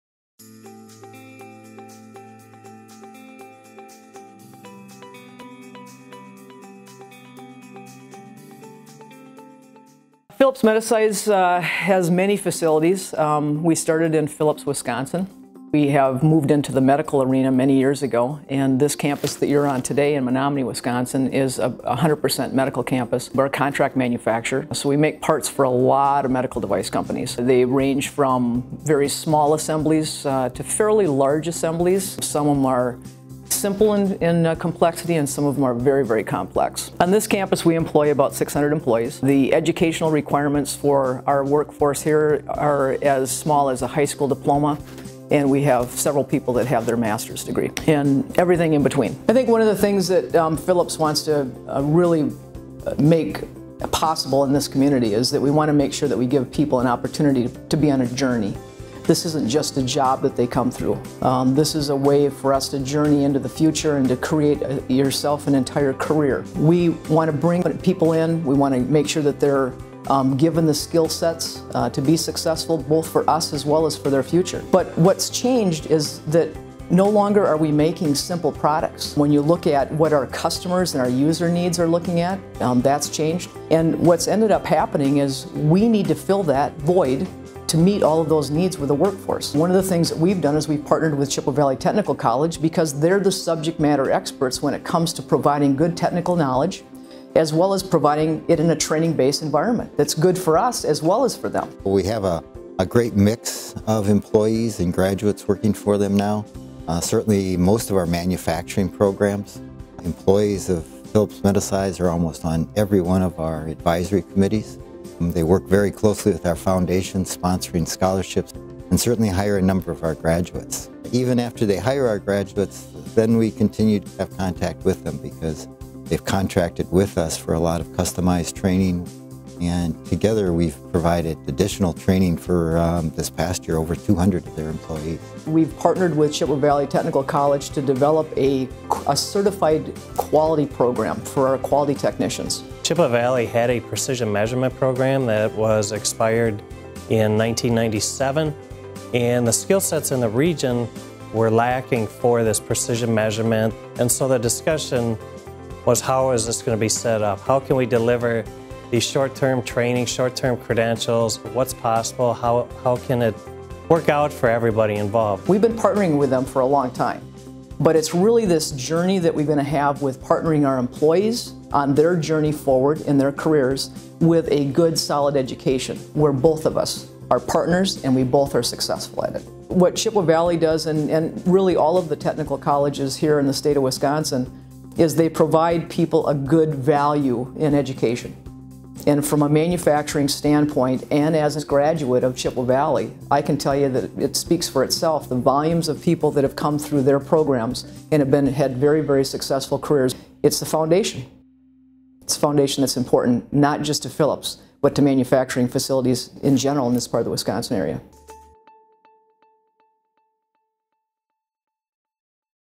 Phillips Medisize An Employer's Perspective
Kim Egan, Quality Manager at Phillips-Medisize, speaks on the value of Chippewa Valley Technical College and the Wisconsin Technical College System.